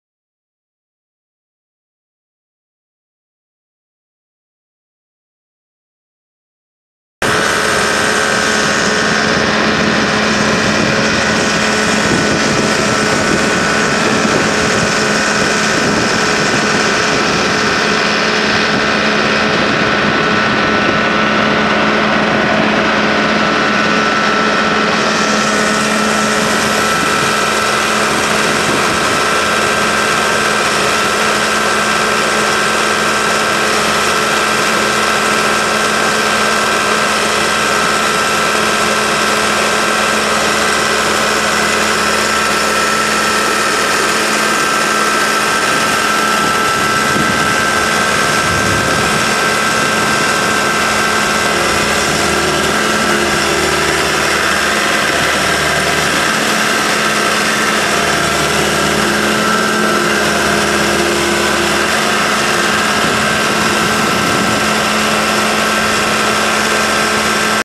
2007 John Deere 650J LGP pipelayer for sale | sold at auction March 10, 2016
SOLD! Manhattan Pipeline and Construction Equipment Auction

2007 John Deere 650J LGP pipelayer
5,312 hours on meter
John Deere 4045T 4.5L six cylinder turbo diesel engine
99 HP
Hydrostatic transmission
OROPS
Joystick steering
Suspension seat
Front and rear work lights
130"W six-way straight dozer blade
Midwestern M520C pipelayer
20,000 lbs lift capacity
20&#039; boom length
27.5"W single grouser track pads
Serial T0650JX149355
Unit# 31-01
Buyer is responsible for all aspects of loadout, no assistance available